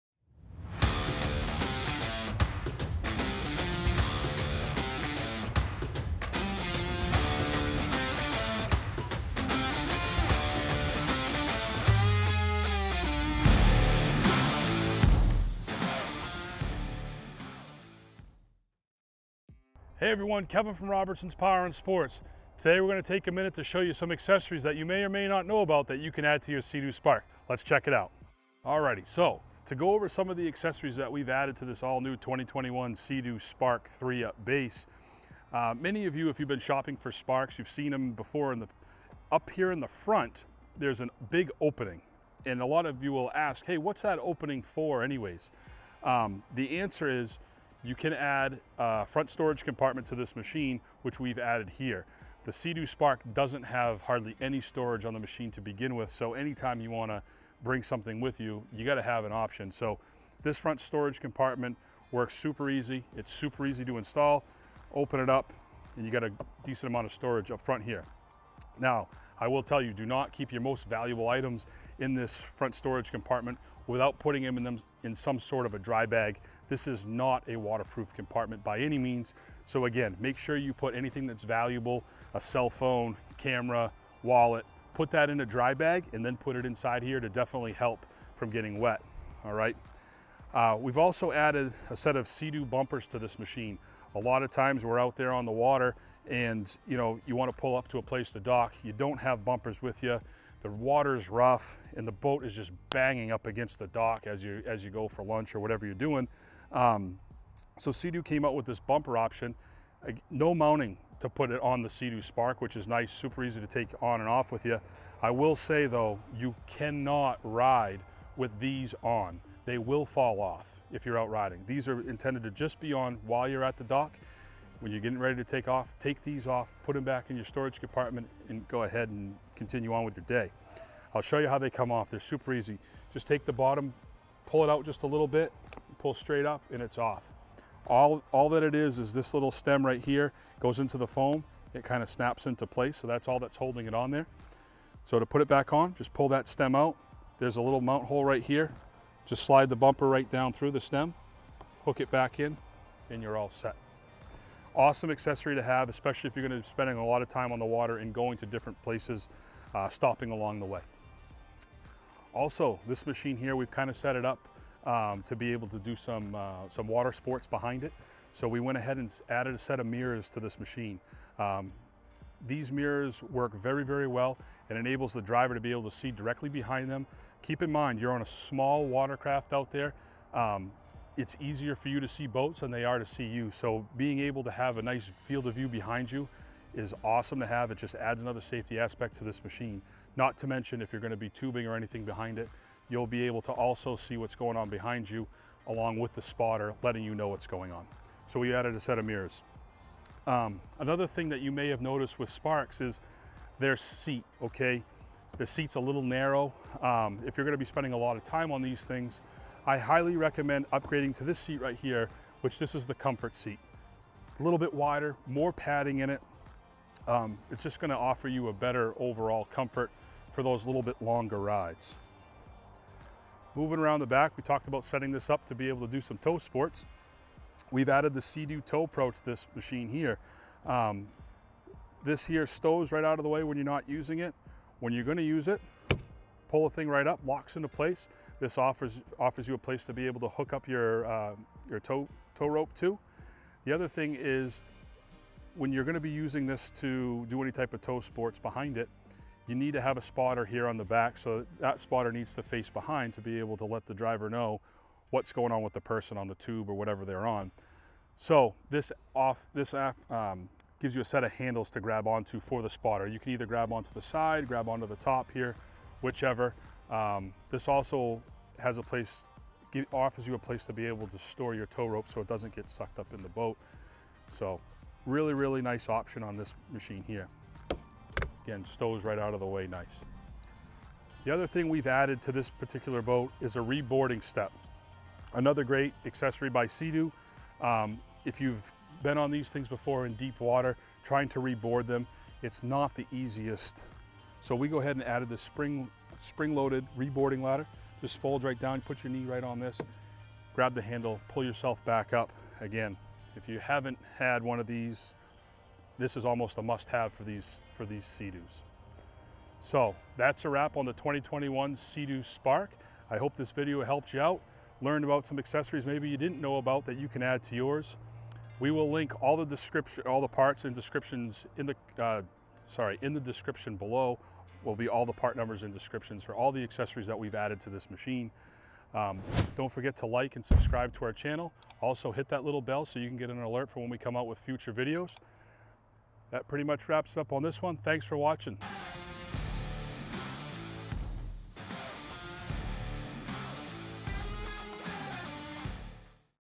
2021 SEA-DOO SPARK 3 UP Walk Around and Accesories
2021 SEA-DOO SPARK 3 UP walk around and accessories!

Walk around of the 2021 SEA-DOO SPARK and accessories!

Accessories used on this Spark

Check out the all new 2021 SEA-DOO 3 UP with some added accessories. See an accessory you like?